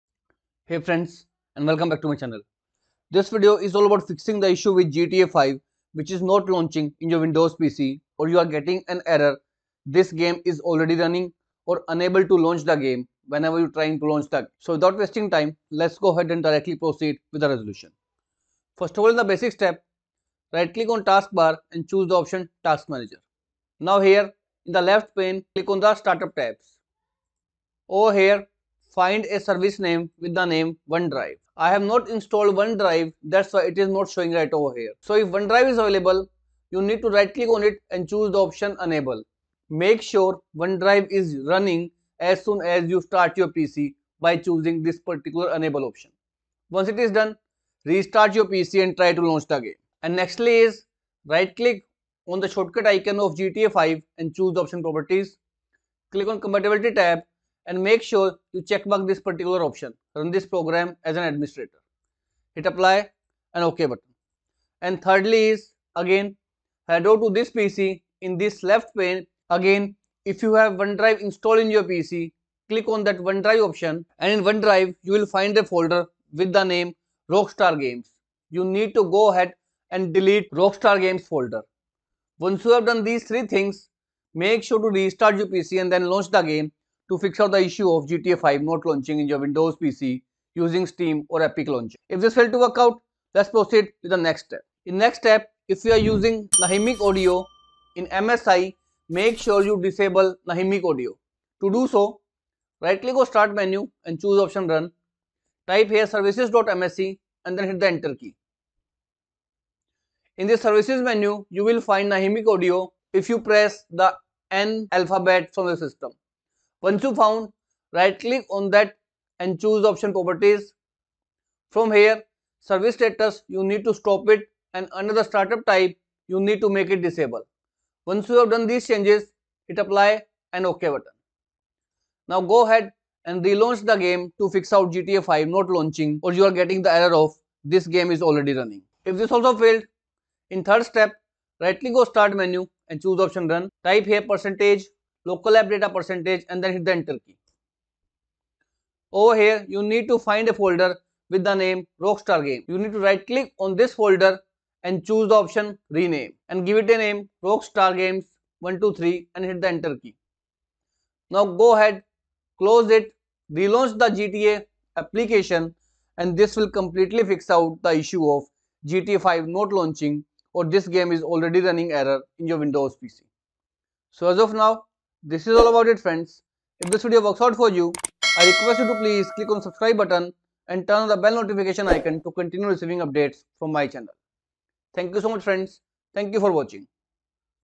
GTA V not launching Fix This game is already running
While launching gta v using epic games or steam the gta v stuck on loading screen or stuck at rockstar games loading screen and then gtv not launching on pc.
So this video is all about fixing gta v not launching on PC or you are getting the error this game is already running in windows PC.
watch the complete tutorial to get the proper resolution for the error in gta v.

00:00 Introduction
00:28 Method 1 using the one drive and few tweaks
01:31 Method 2 by disabling nahimic audio
02:18 Method 3 using local games folder